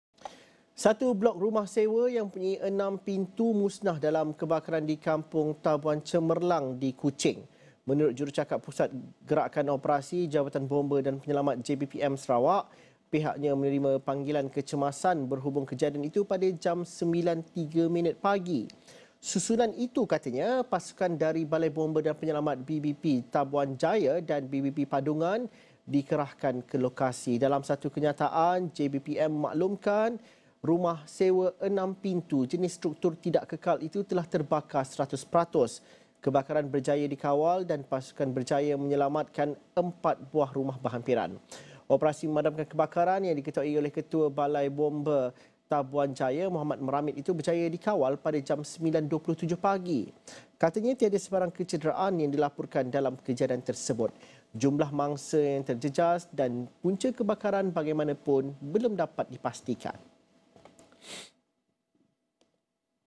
Rumah sewa enam pintu musnah
Satu blok rumah sewa yang mempunyai enam pintu musnah dalam kebakaran di Kampung Tabuan Cemerlang, Kuching.